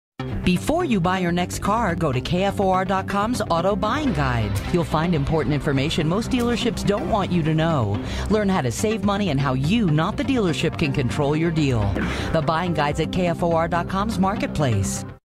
Promo-KFOR Marketplace 01
This commercial was edited by J.C. Bowers for KAUT/KFOR in Oklahoma City. This promotion may no longer be valid.  J.C. is now the owner of Campfire Creative, LLC, an independent commercial production company, also in Oklahoma City.